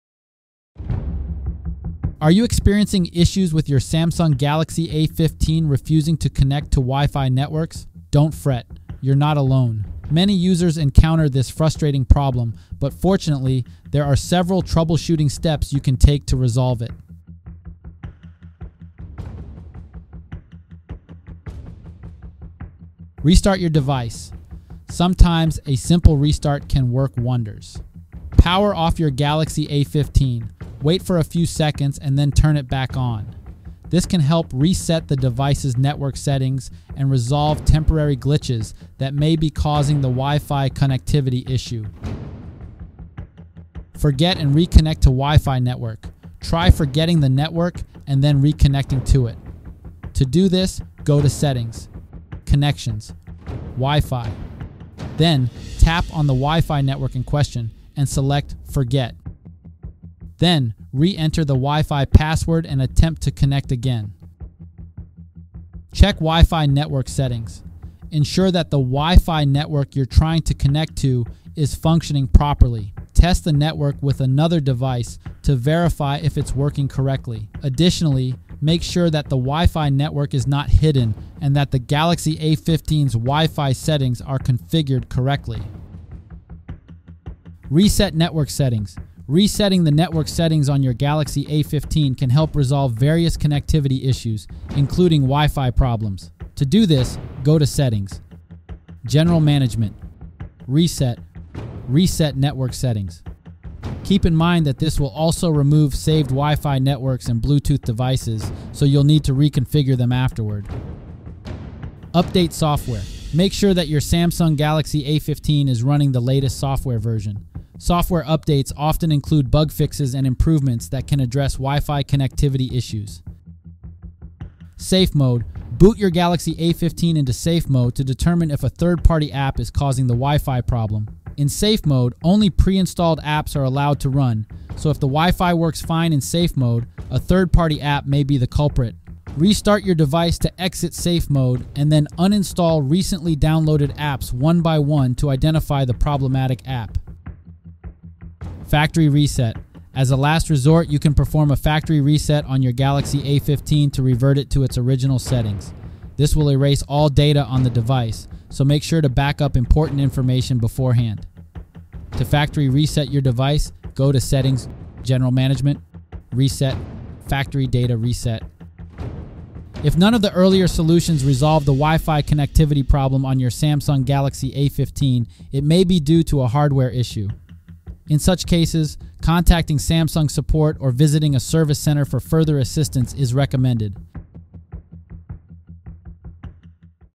How to Fix Samsung Galaxy A15 Won't Connect to Wi-Fi
Are you struggling with Wi-Fi connectivity issues on your Samsung Galaxy A15? You're not alone! In this comprehensive video guide, we'll walk you through a step-by-step process to troubleshoot and resolve Wi-Fi problems on your Galaxy A15.

We'll cover various solutions, including restarting the device, forgetting and reconnecting to the Wi-Fi network, checking Wi-Fi network settings, resetting network settings, updating the software, booting into Safe Mode, and performing a factory reset.

Don't let Wi-Fi connectivity issues ruin your mobile experience. Watch our video guide today and get your Galaxy A15 back up and running smoothly!